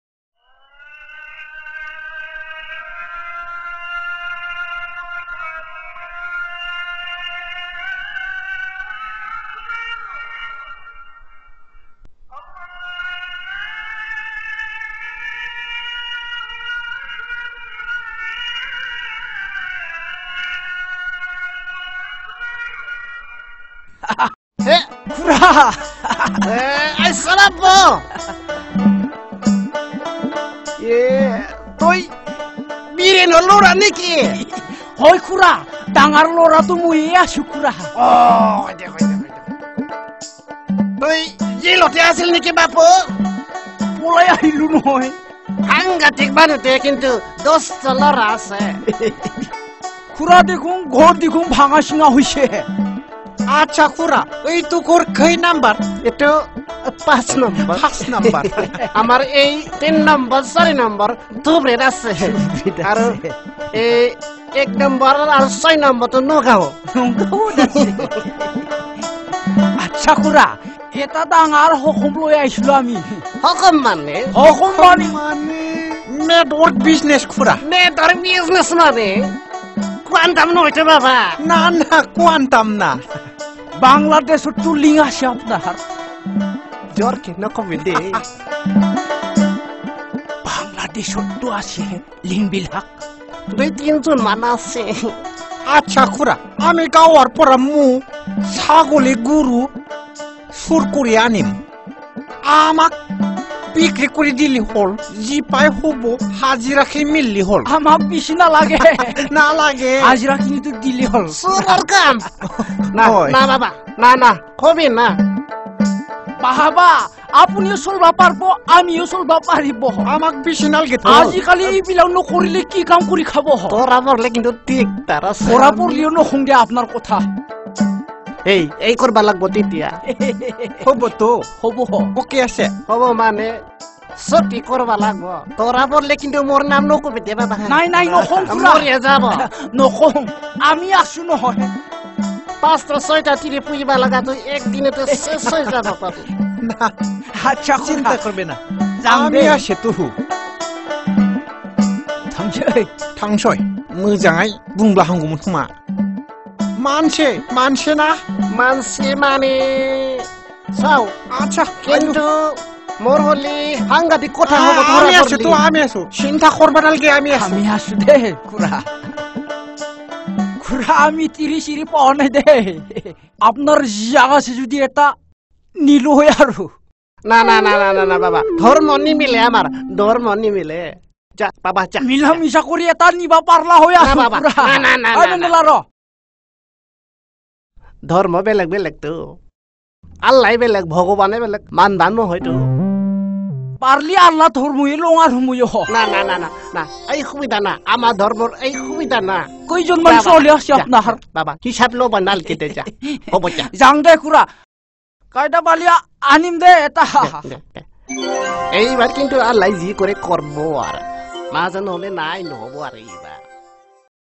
Hainamuli 4(Network).mp4
Mendela, Phwila  and Ali discussing to export cattle in Bangladesh.